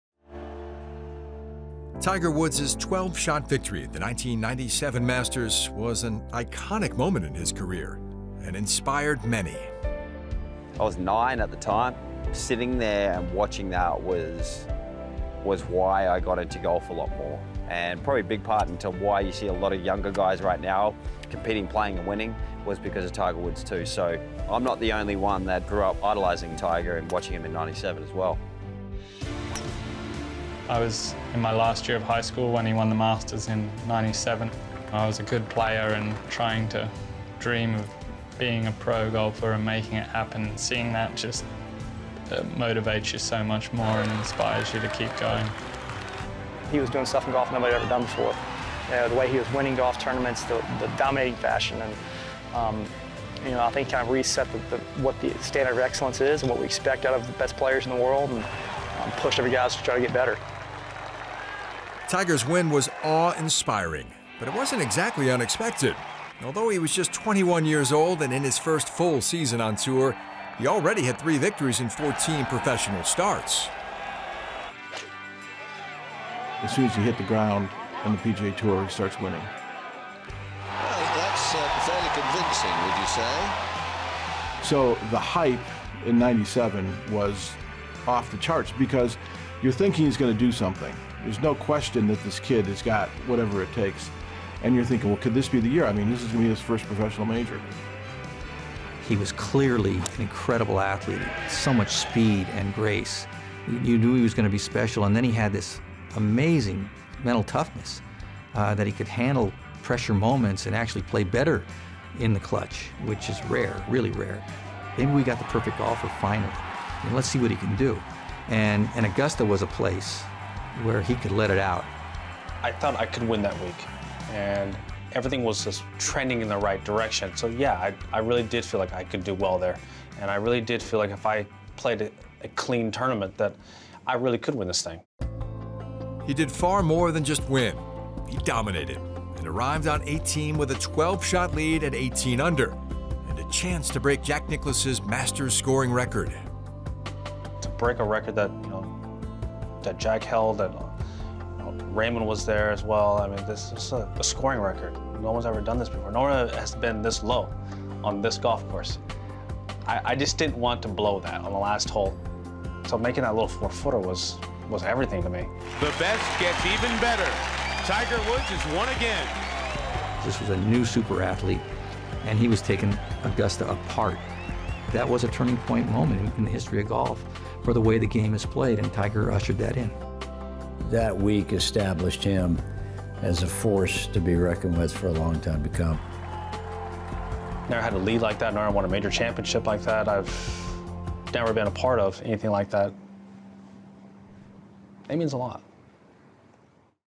Tiger Woods’ iconic 1997 Masters victory
Tiger Woods’ 12-shot victory at the 1997 Masters was a captivating moment in golf history. The 21-year-old phenom shattered multiple scoring records on his way to his first career green jacket.